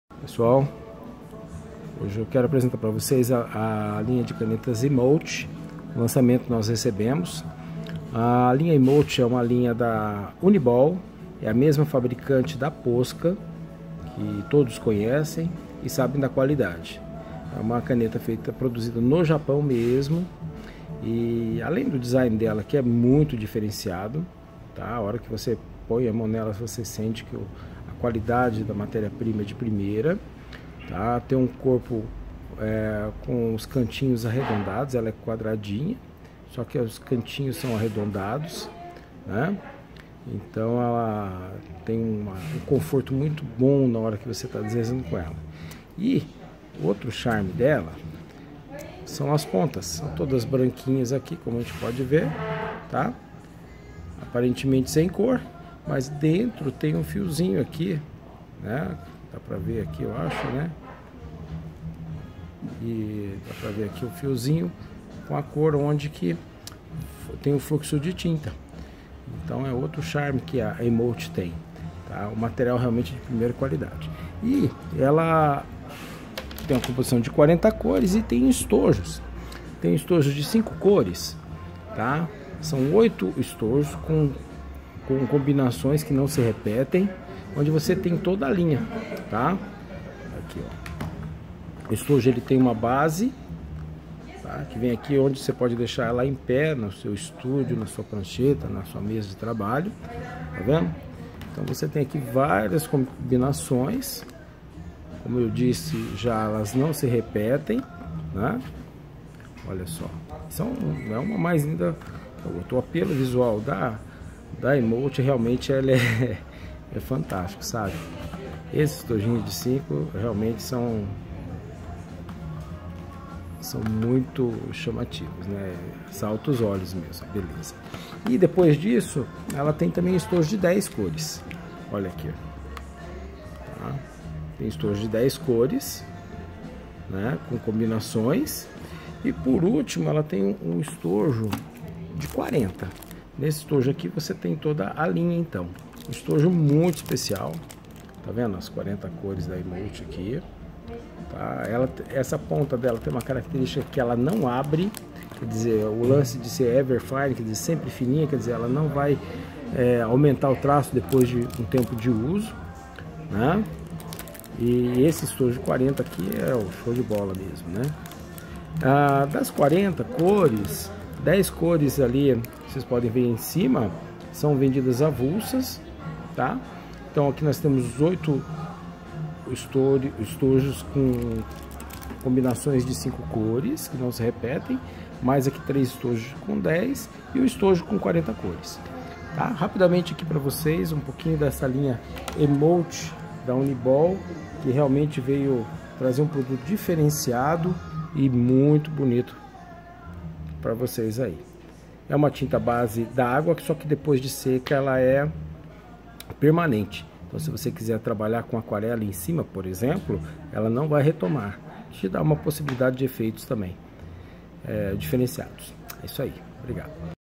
Canetas Emott by Uniball
Caneta Uni Emott, a caneta com a super ponta fina é duravel resistente a luz, a largura de sua linha é de 0.4mm e permaneçe constante.
Desing moderno e ergonomico, com a tampa que é segura e facilmente acoplavel. Disponivel em 40 cores.
Tinta resistente a água.